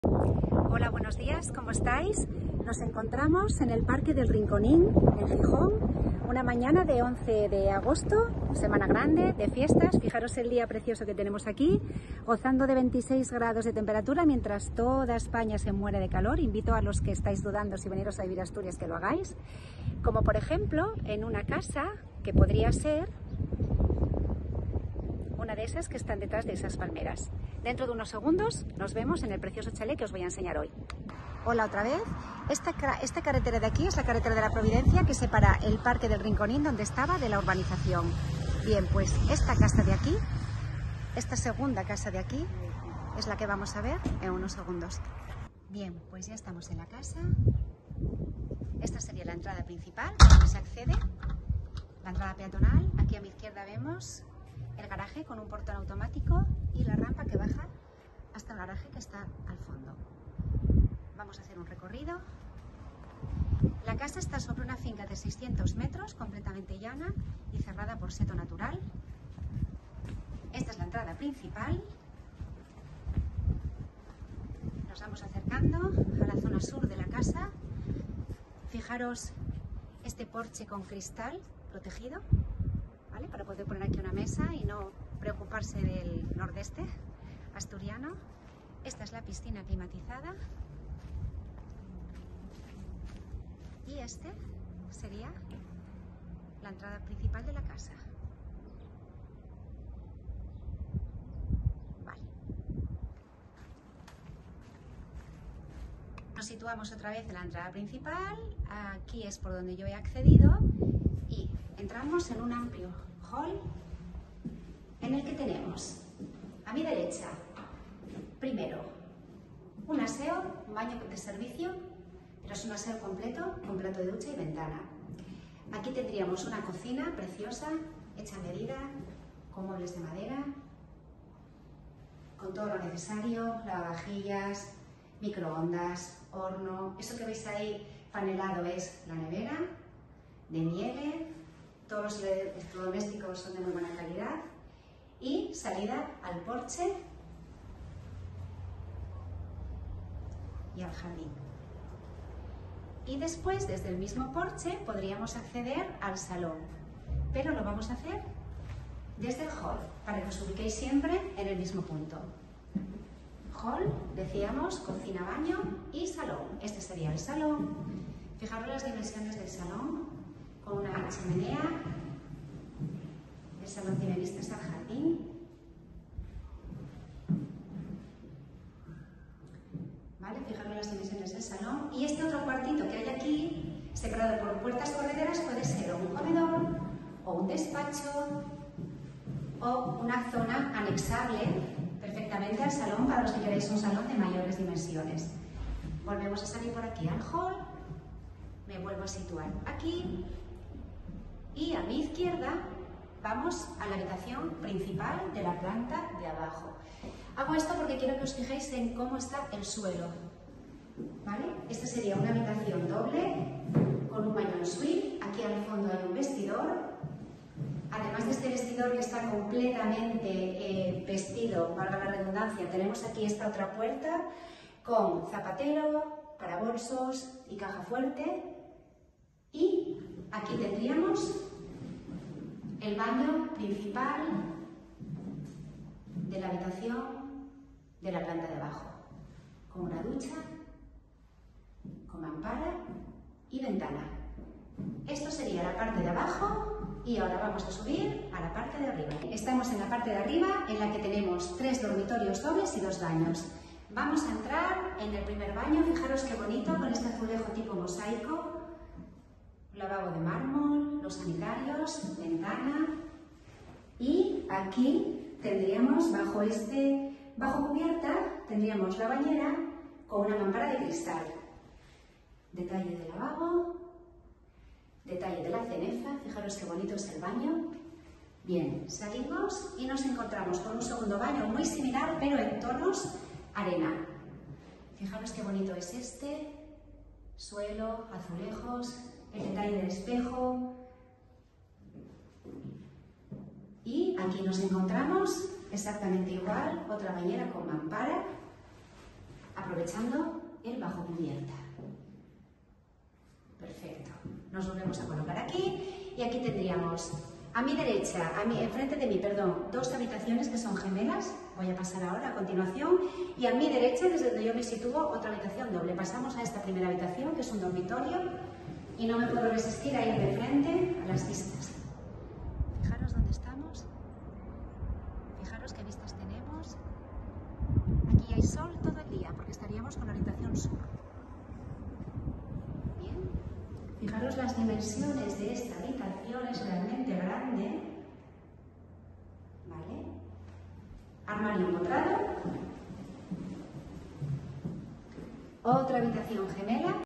🤝VENDIDO 🤝Chale con piscina Somió a 200 m de la playa. 750.000€ Gijón
Nos encontramos ante una preciosa casa independiente sobre una finca de 600 metros con piscina y bonitas vistas.
La casa se encuentra en una de las mejores zonas de Somio por su cercanía a la playa a la que se llega caminando en a penas 5 minutos.
Se trata de una casa del año 2006 y en excelente estado de conservación que consta de 5 habitaciones, 3 baños completos y dos aseaos, un garaje todo ello dividido en dos plantas y sótano
Calidades de lujo, los gastos de comunidad no llegan a 60€
No dudes en contactar si te es lo que estás buscando porque te sorprenderá.